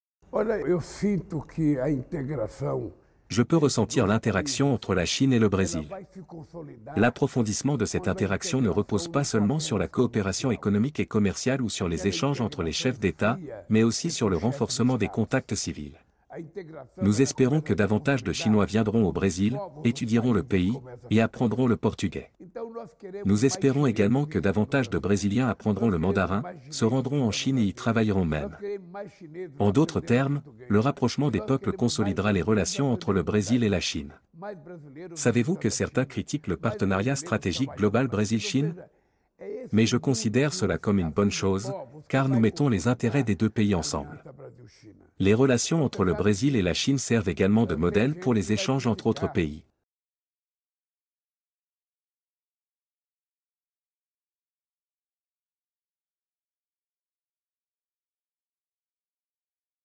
Président brésilien : les échanges humains consolideront les relations Brésil-Chine
Les échanges culturels et humains entre la Chine et le Brésil se sont intensifiés dans divers domaines. De plus en plus de Chinois s’enthousiasment pour la culture brésilienne, en particulier pour le football, la samba, le barbecue, le jiu-jitsu et le cocktail Caïpirinha. En contrepartie, les éléments chinois sont devenus aussi populaires auprès du peuple brésilien. Dans une interview accordée à CMG, le président brésilien, Luiz Inacio Lula da Silva, a fait part de son point de vue sur l'approfondissement continu des échanges entre les deux peuples.